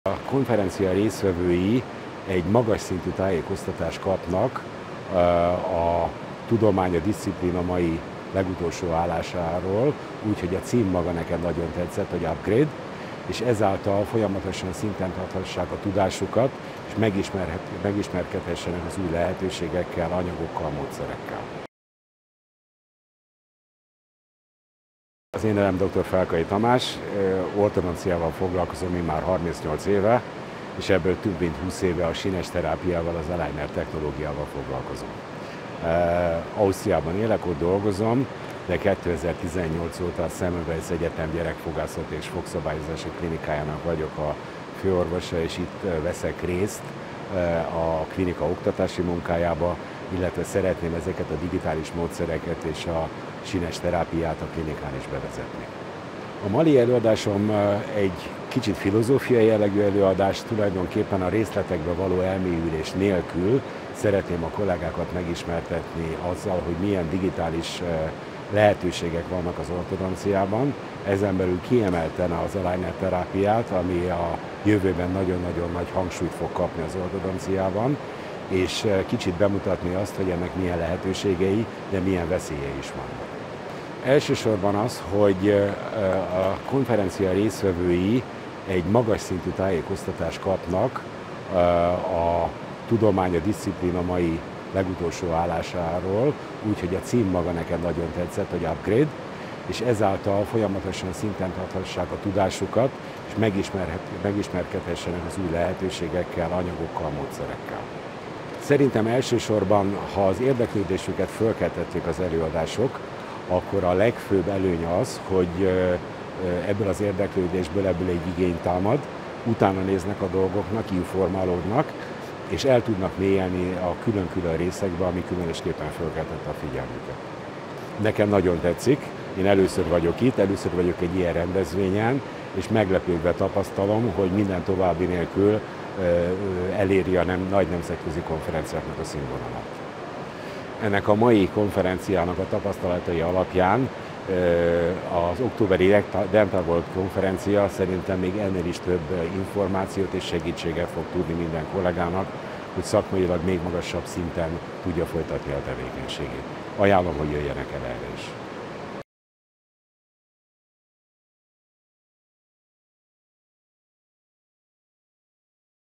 Dr. Felkai Tamás - Upgrade Congress 2023
Dr. Felkai Tamás szerint a konferencia résztvevői olyan nagyfokú tájékoztatást kapnak, aminek segítségével még magasabb szakmai szinten tudják majd folytatni munkájukat a rendelőben.
📍 Dental World - október 12-13-14, 2023
📍 Upgrade Congress - április 19-20, 2024